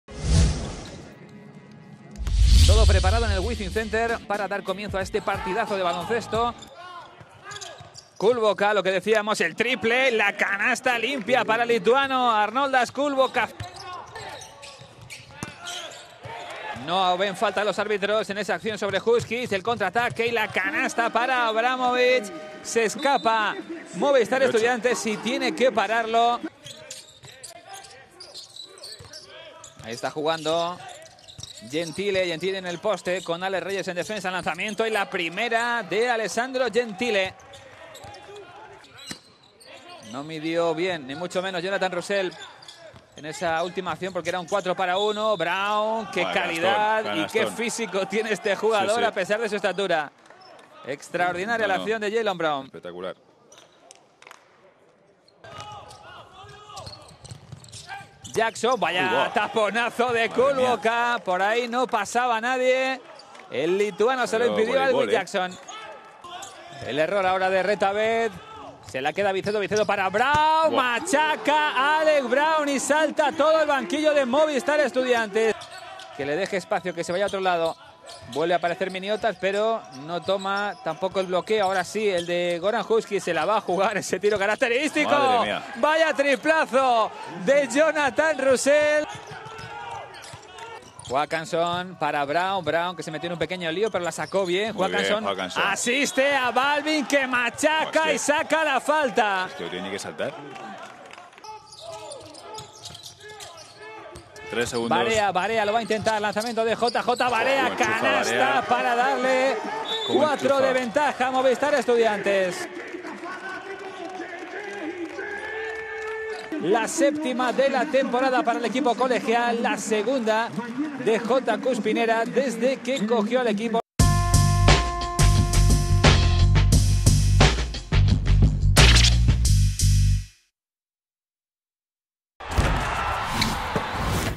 Movistar Estudiantes - RETAbet Bilbao Basket (95-89) RESUMEN | Liga Endesa 2020-21
Un coral Movistar Estudiantes se impuso por 95 a 89 a RETAbet Bilbao Basket en un duelo que aleja a los colegiales de la zona peligrosa de la tabla tras llevarse los dos encuentros contra un rival directo. JJ Barea, Avramovic, Roberson, Brown y Arteaga se repartieron los puntos para superar a un conjunto bilbaíno que siempre fue a remolque.